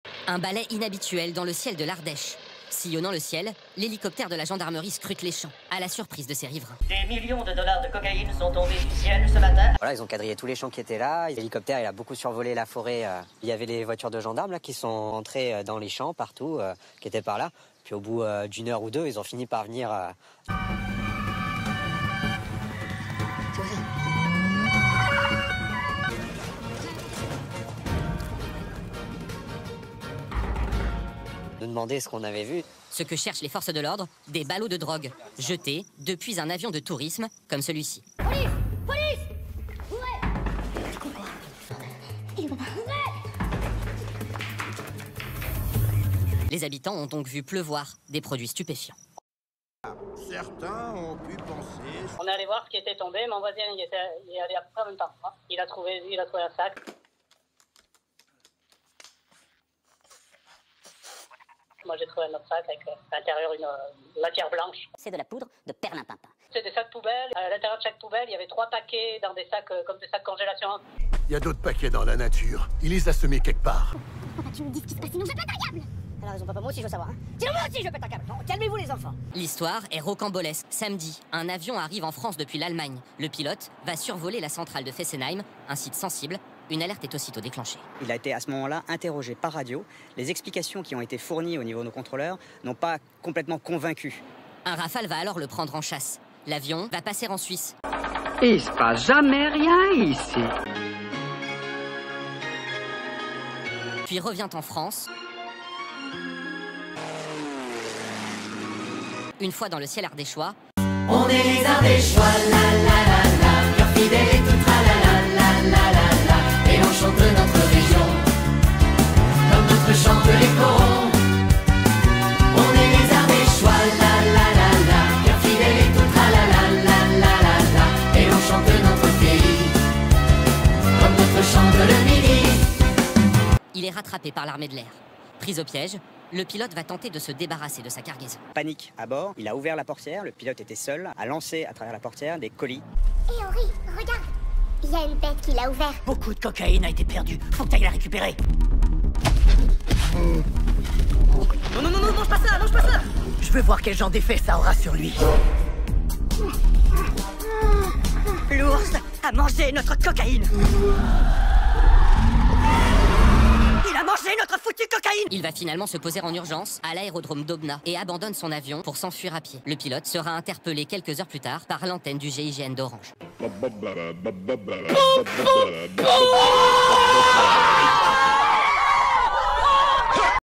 L'Affaire de l'Avion Drogue en Ardèche - Vidéo Délire#delire
Infos délire! Plongez dans cette vidéo délirante où un avion transportant de la drogue lâche sa cargaison en survolant l'Ardèche ! Avec une touche d'humour et un montage créatif, découvrez cette histoire incroyable qui a captivé l'attention de tous. Une aventure rocambolesque qui montre que la réalité peut parfois dépasser la fiction.